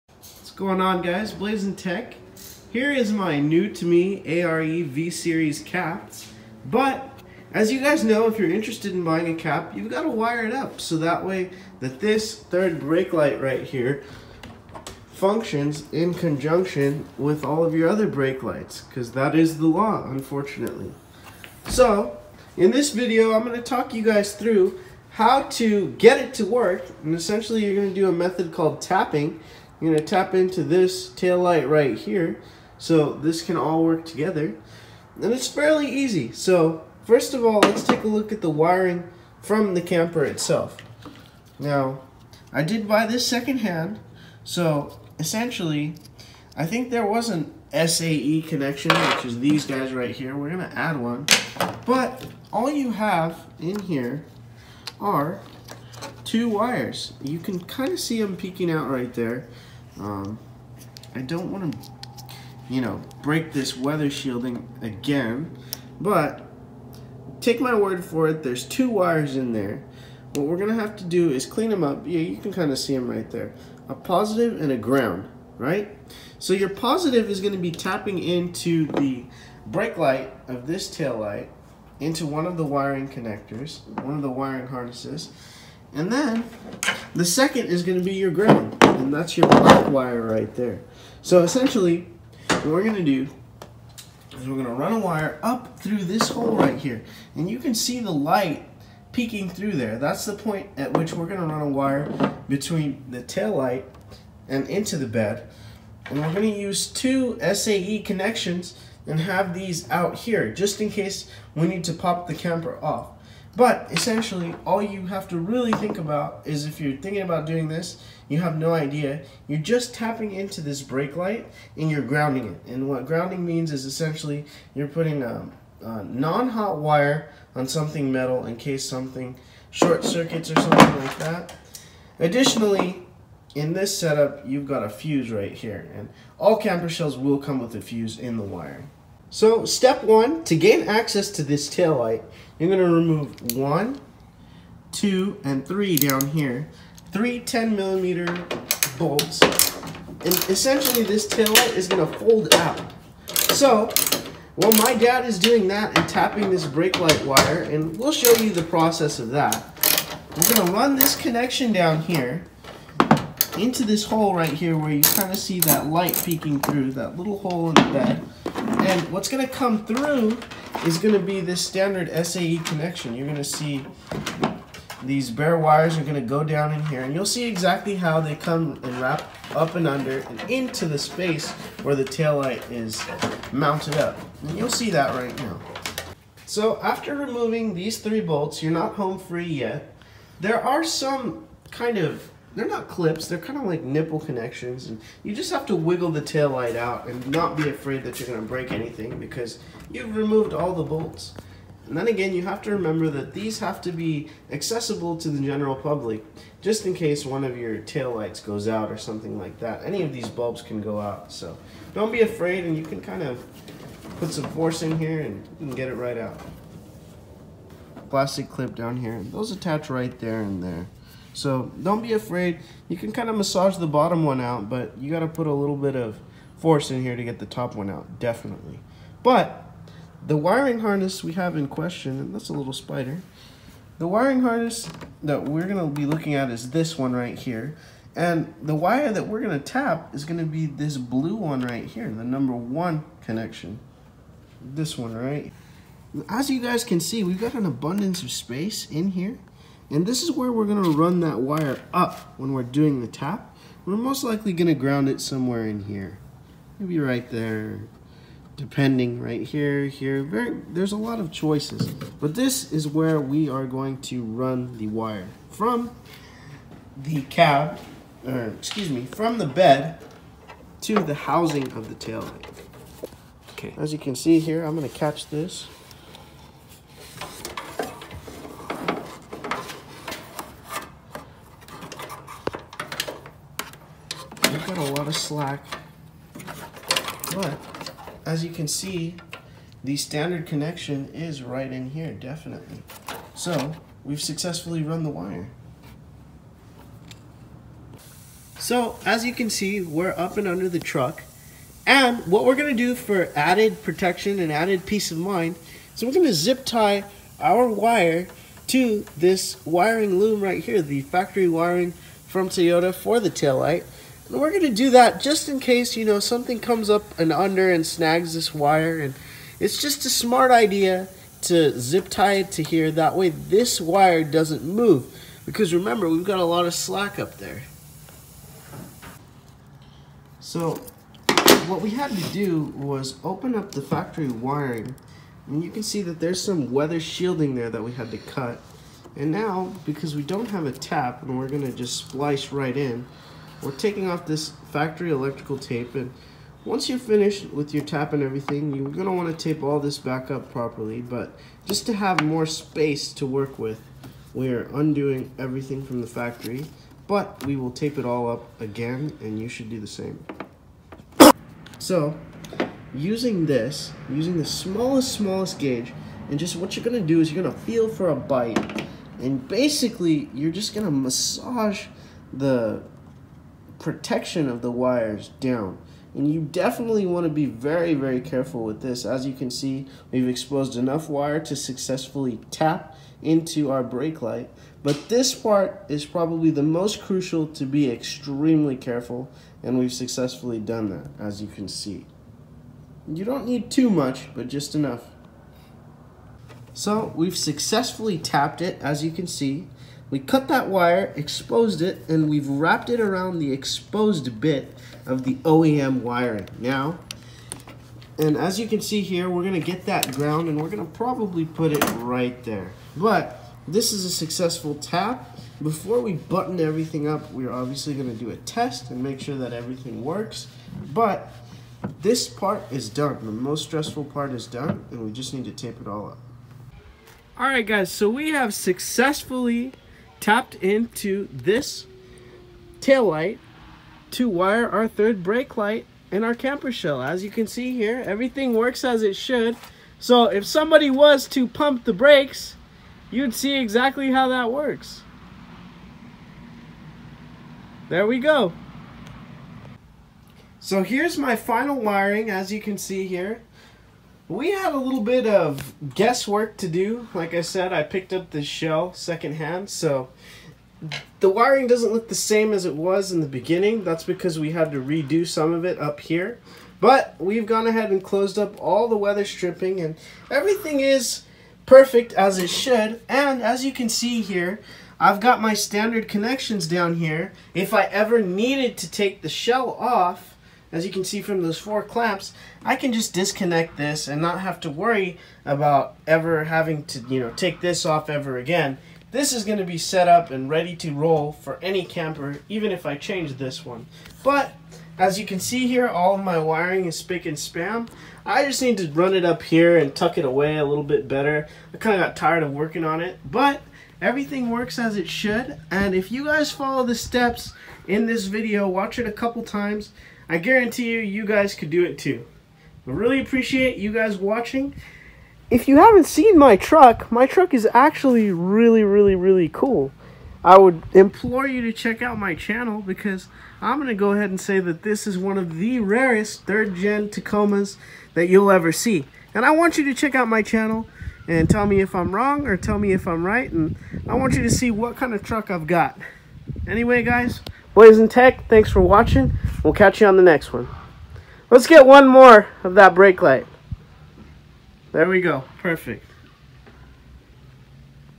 2016-20 Tacoma Camper (3rd Brake Light) Wiring Tutorial
This is a step by step guide on how to tap the oem wiring for the tail light on a 3rd Gen Tacoma in order to power a brake light. This is specifically used for a camper shell, but the process will be the same for a trailer or anything else.